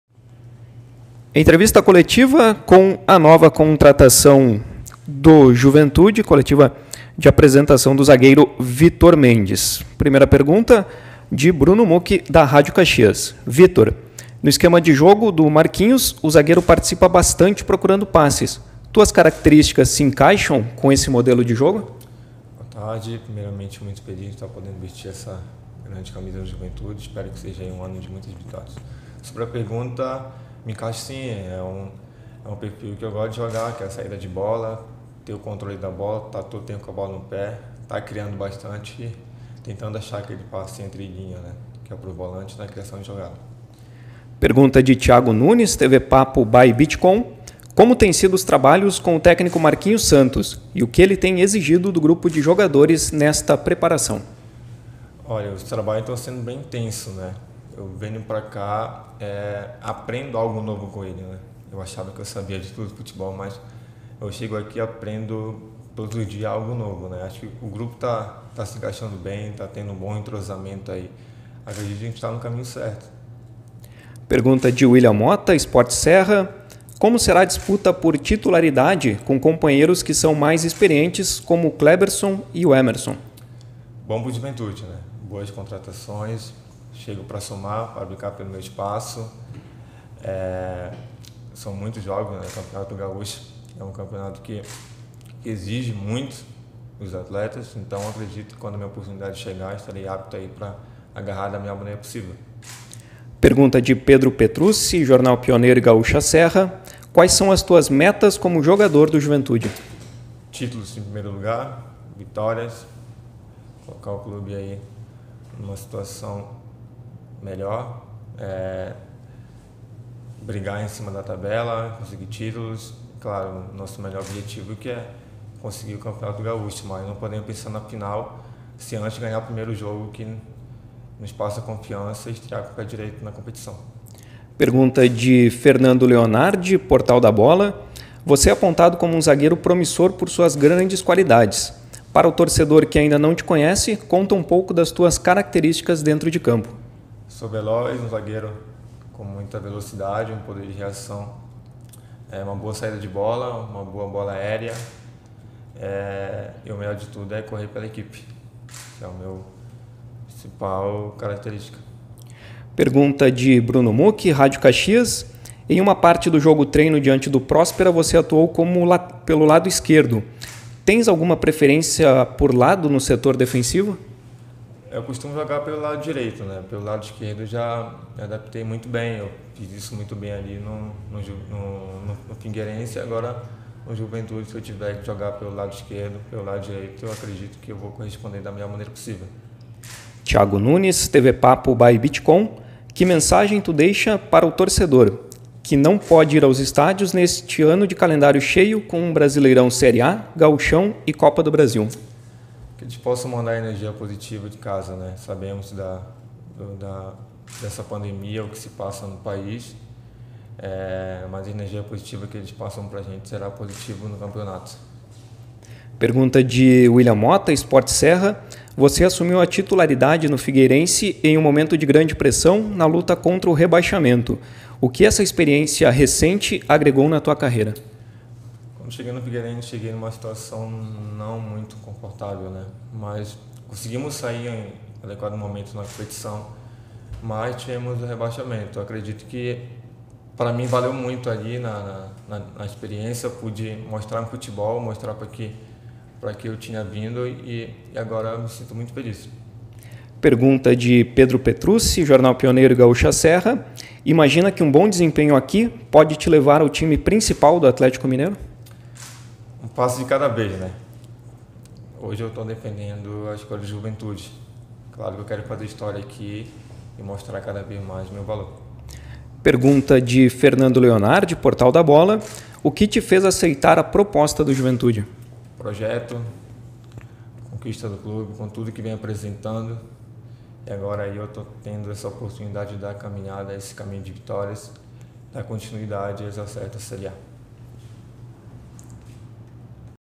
Apresentação zagueiro Vitor Mendes
O zagueiro Vitor Mendes foi apresentado como novo reforço do Juventude para a temporada 2021. O jogador concedeu sua primeira entrevista coletiva vestindo a camisa do clube na segunda-feira (22.02).

O defensor pertence ao Atlético-MG e no último Campeonato Brasileiro da Série B atuou pelo Figueirense-SC. Também defendeu Guarani-SP e Boa Esporte-MG.

Torcedor Jaconero, jogue JUnto do Juventude no projeto "Iluminar o Jaconi".

As perguntas foram enviadas por profissionais da imprensa e repassadas ao jogador pela assessoria de imprensa do clube.